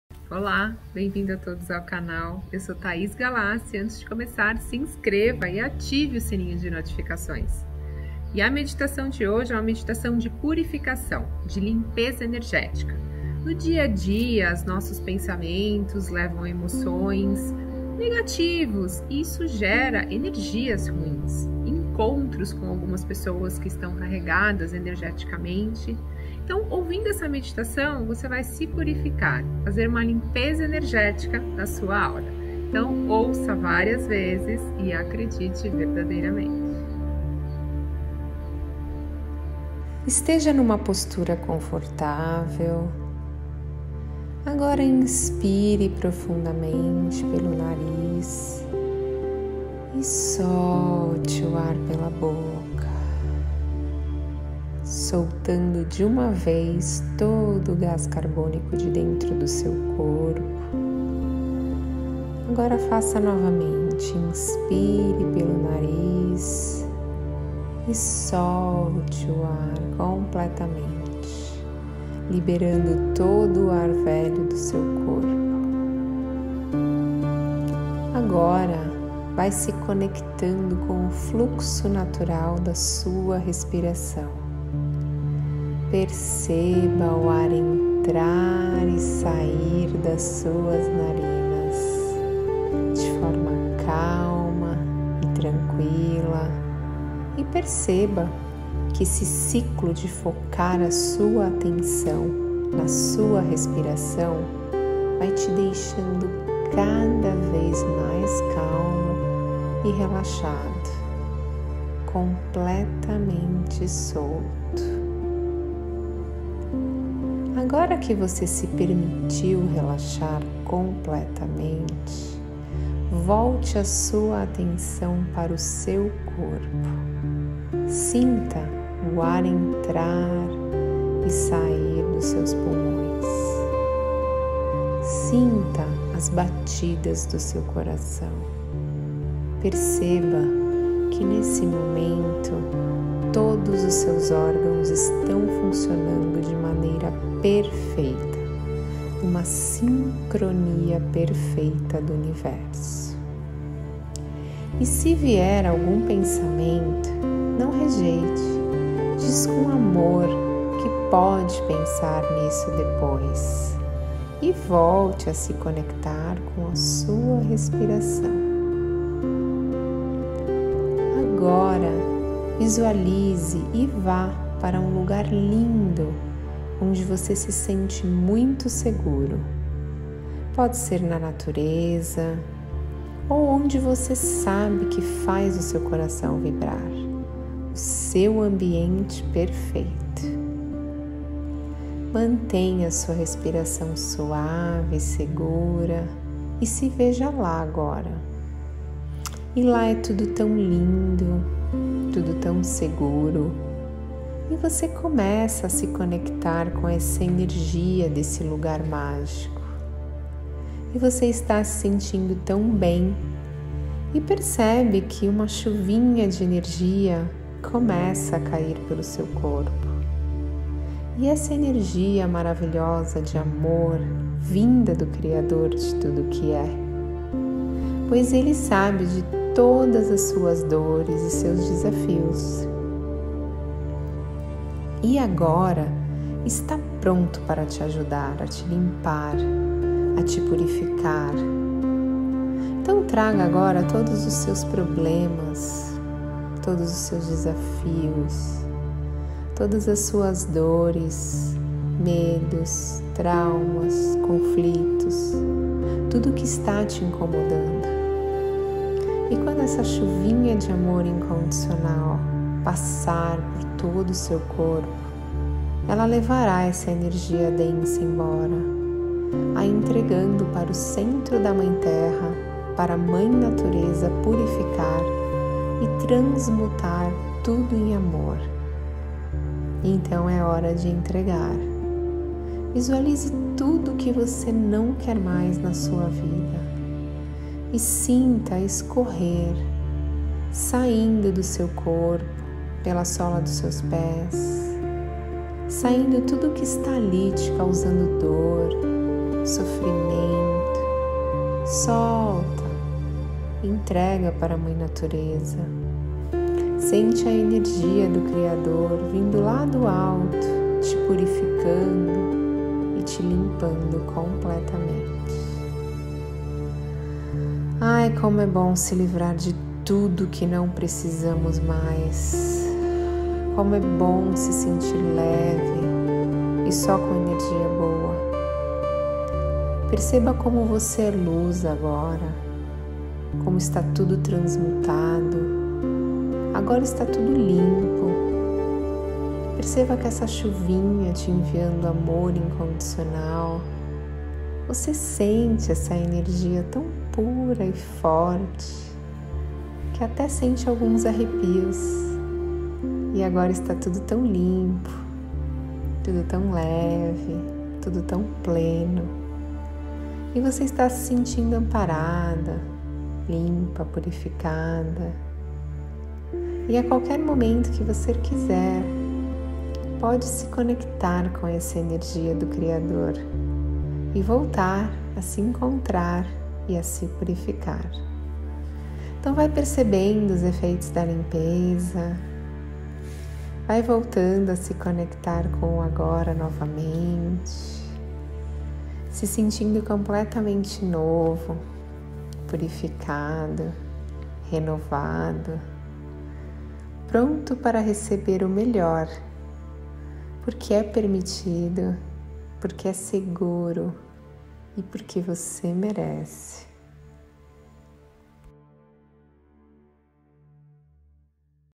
Meditação de Limpeza Energética.
No dia a dia acabamos absorvendo energias pesadas de outras pessoas ou lugares, e o resultado disso é um desequilíbrio mental, emocional e energético.
Quer manter sua imunidade alta? Estar bem todos os dias e com muita disposição?
Ouça essa meditação todos os dias.